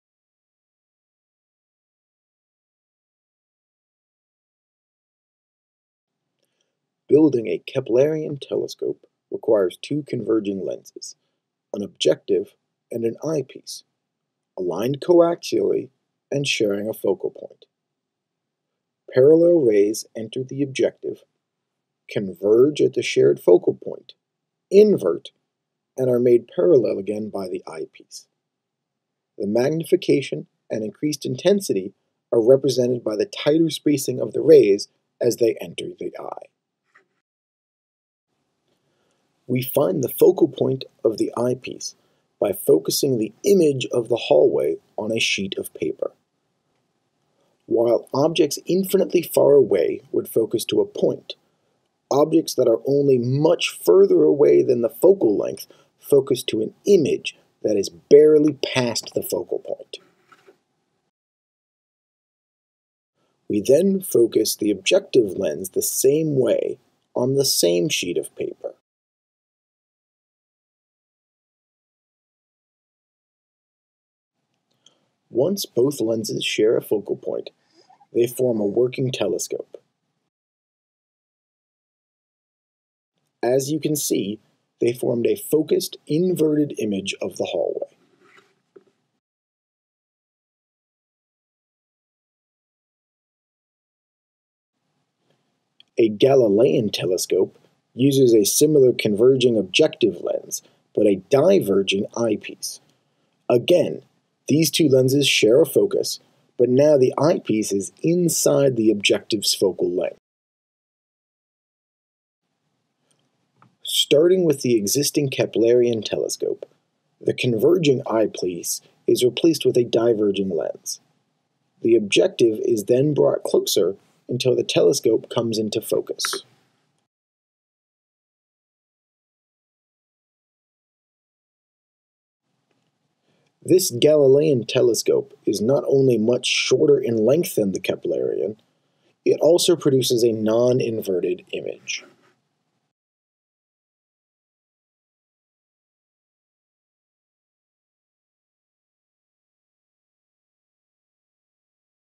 Lenses Lab: Building Telescopes
This video was made for the Refraction and Thin Lenses Lab for PHYS 102 and PHYS 151.
A Keplerian and Galilean telescope are constructed on a table top using simple lenses.

The video begins a ray optics explanation of A Keplerian telescope. A pair of converging lenses are then focused on a shared point to create a working telescope. After a ray optics description of a Galilean telescope, the converging eyepiece of the telescope is replaced by a diverging lens. The length of the telescope is then adjusted until it becomes a working telescope again.